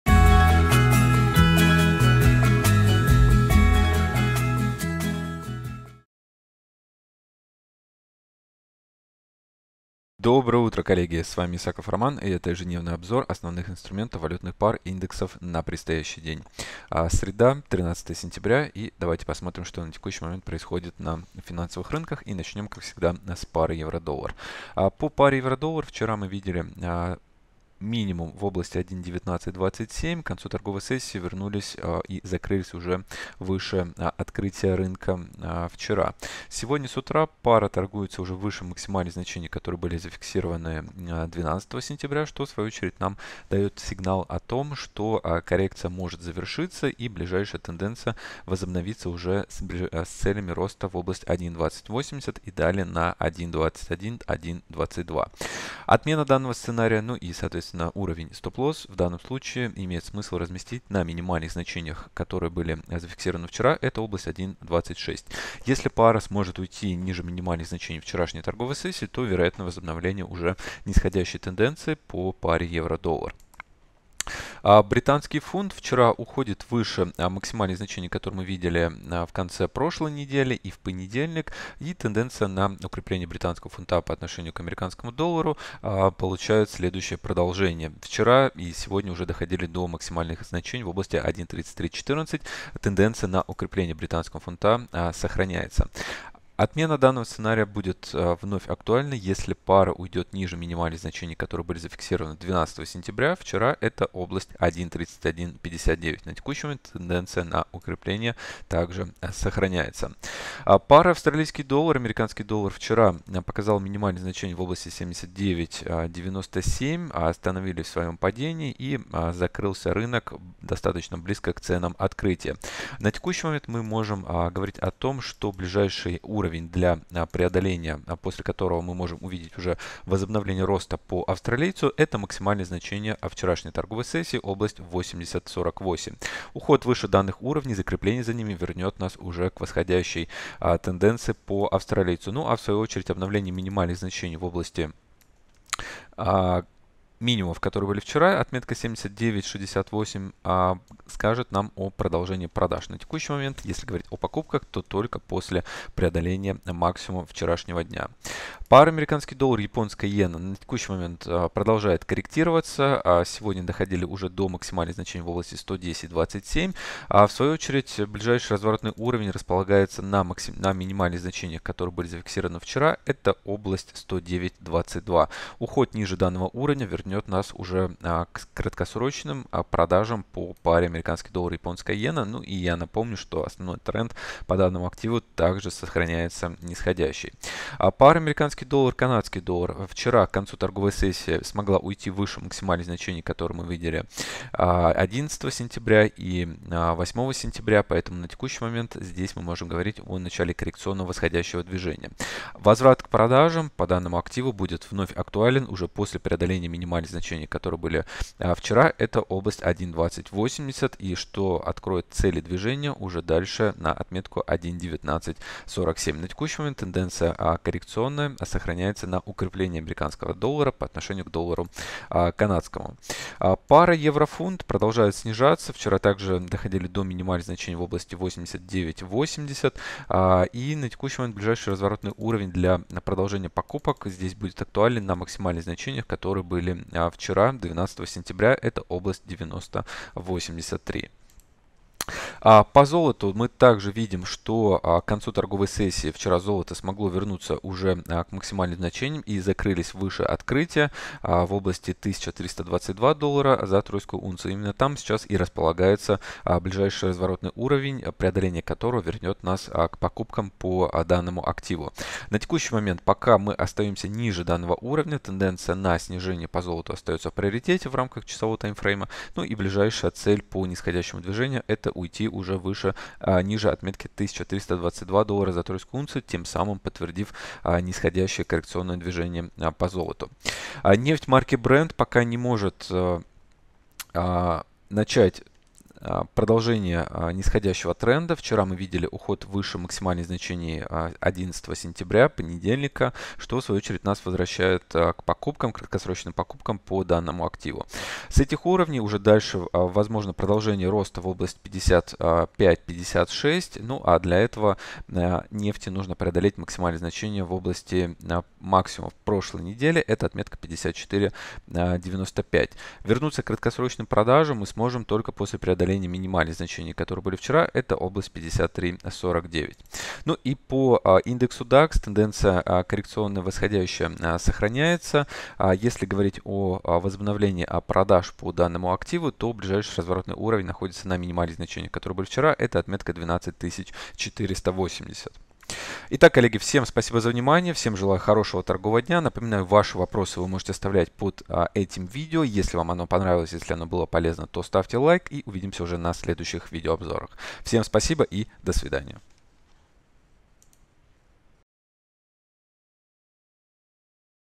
Прогноз рынка форекс на основе системы Price Action с Романом Исаковым 13 сентября 2017
Ежедневный обзор текущей ситуации на финансовых рынках